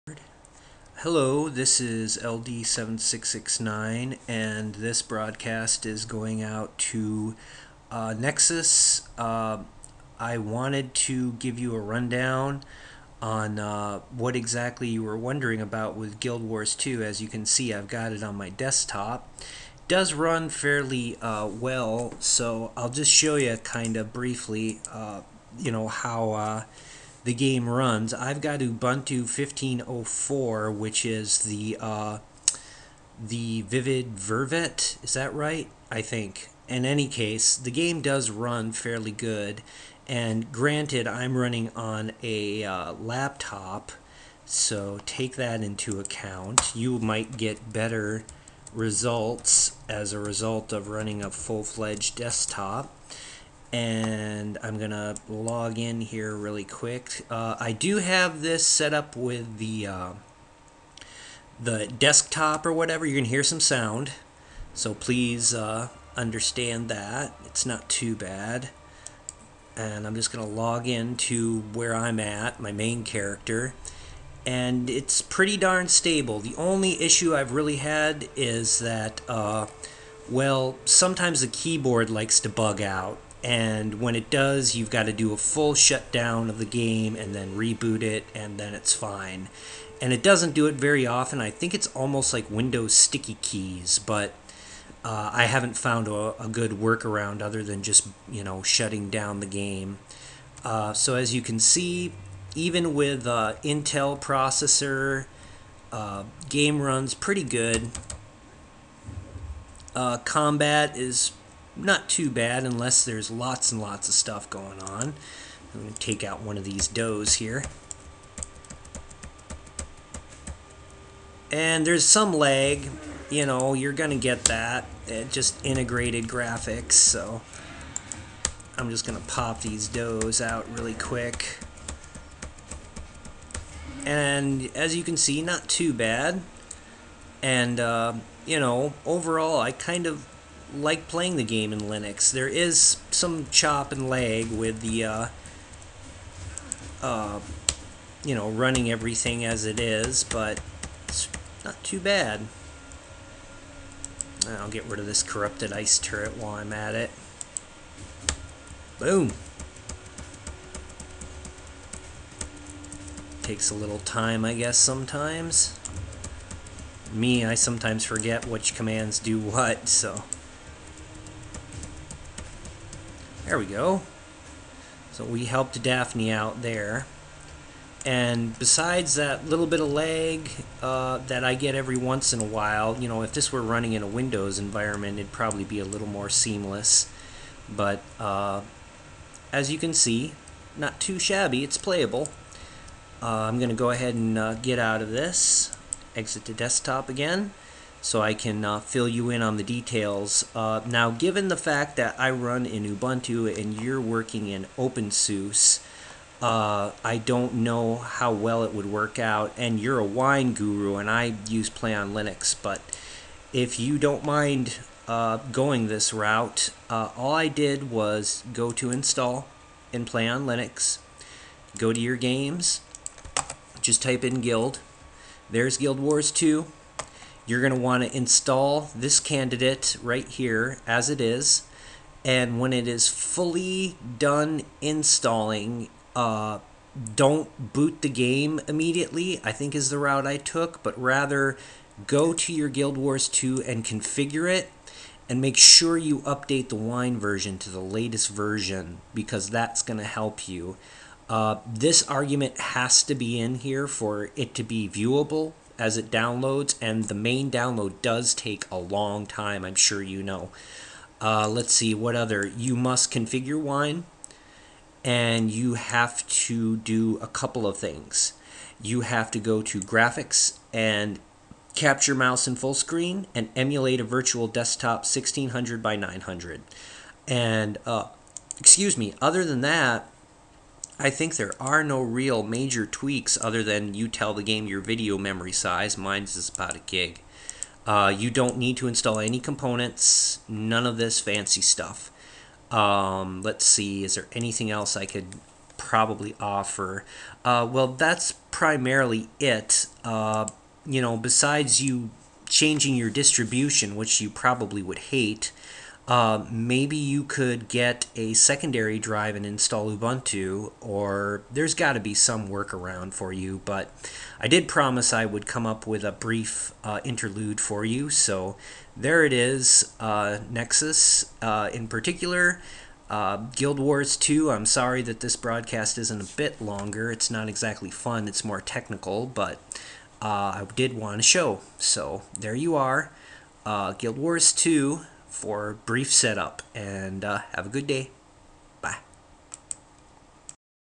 Guild Wars2, installation on Play On Linux, Ubuntu 15.04
Guild Wars 2 installation.  Ubuntu 15.04 Vivid Vervet version, with POL.